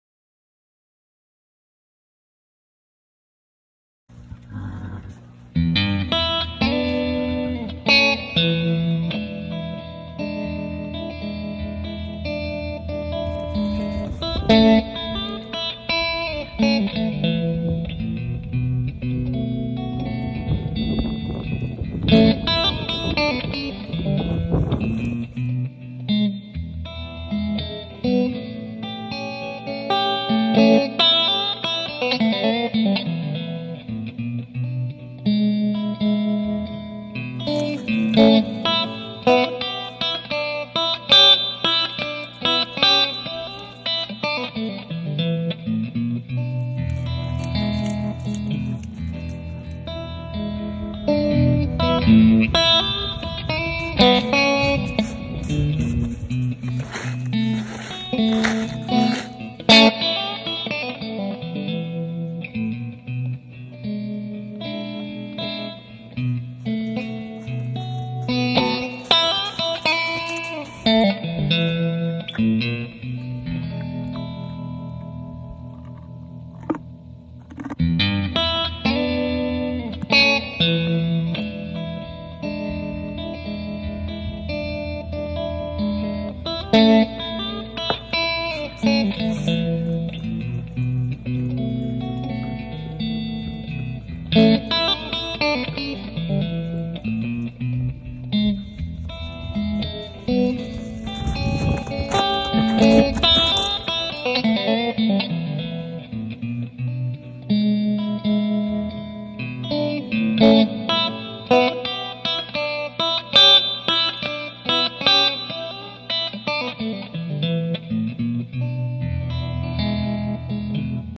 Winter Wanders HD 720p
The adventures of Frodo, my siberian husky, and Jackson, my arabian horse, continue in this video.